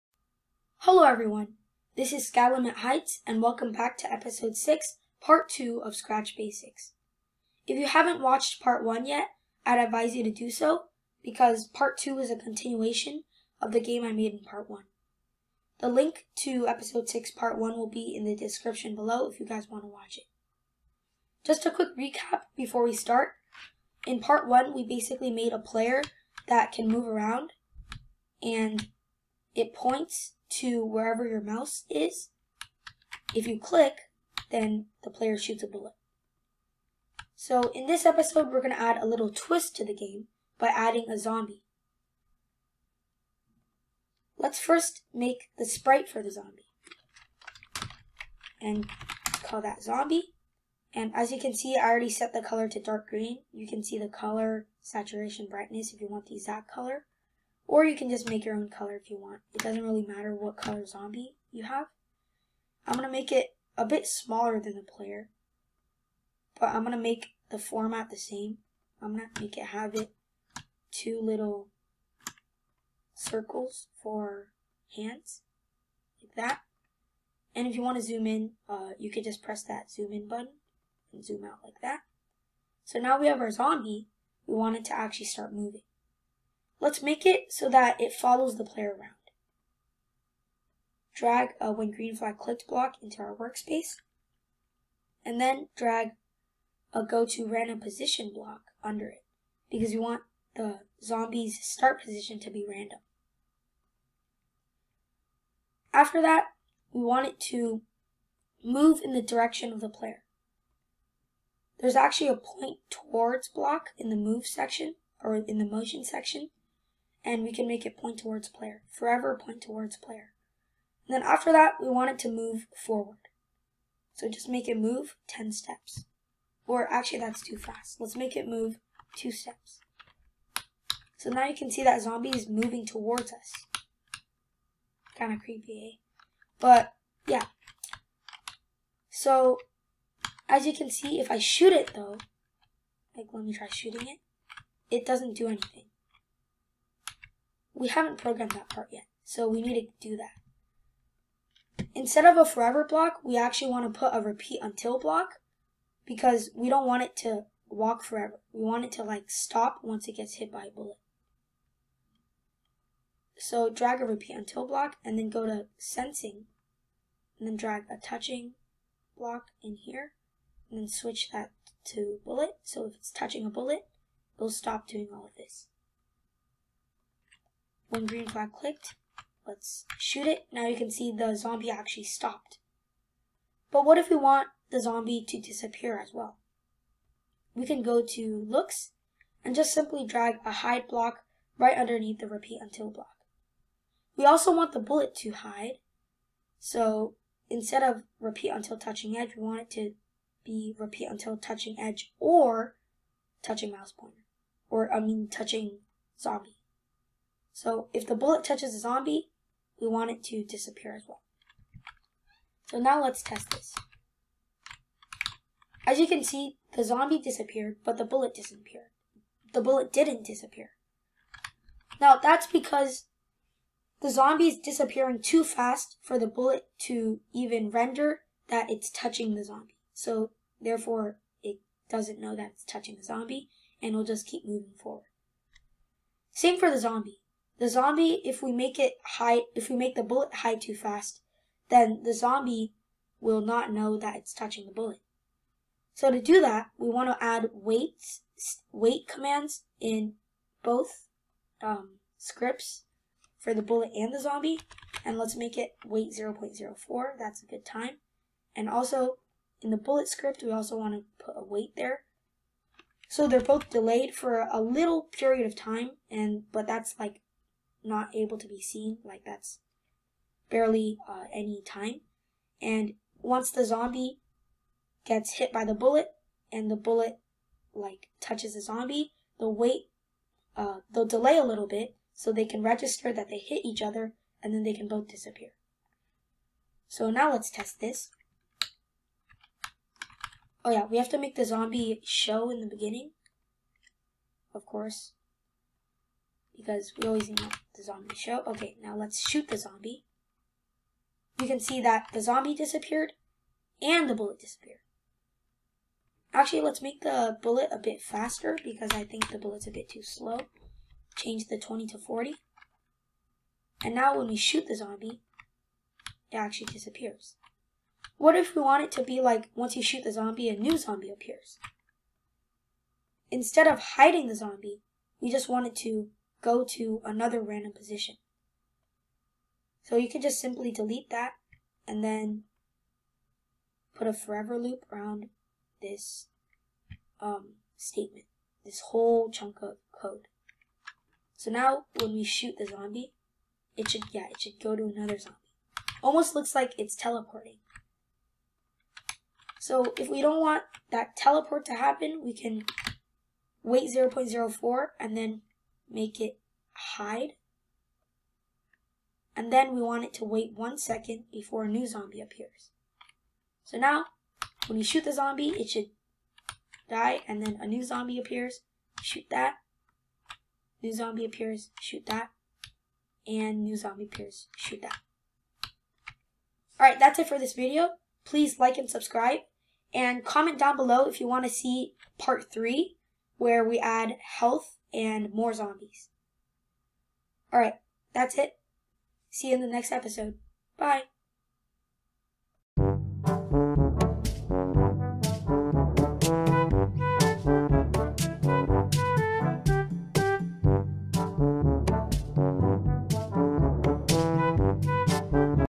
Scratch Basics(Ep. 6, Pt. 2) - Zombie Shooter Basics
In today's video, we expanded our Zombie Shooter Game by adding a moving zombie that follows you around. When this zombie is shot, it dies and a new zombie appears in another random location on the map!